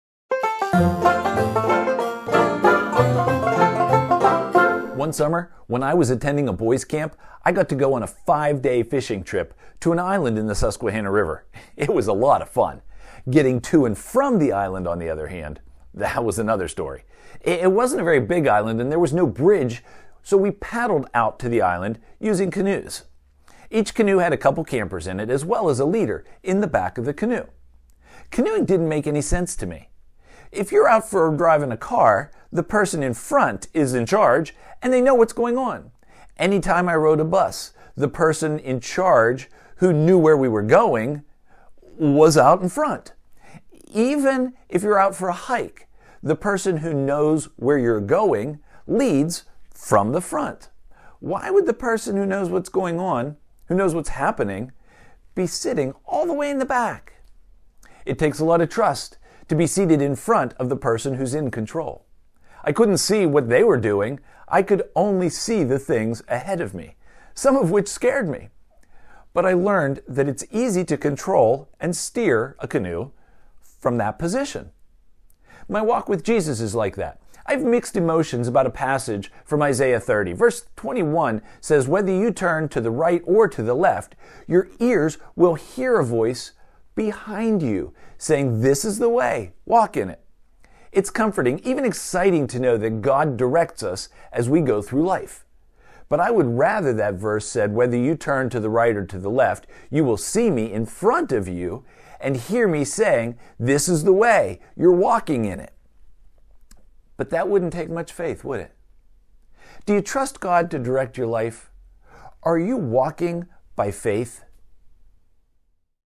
Canoeing
Sometimes we don't know where we're going and it feels like we're on our own. But Jesus is ALWAYS guiding us - even when we can't see Him!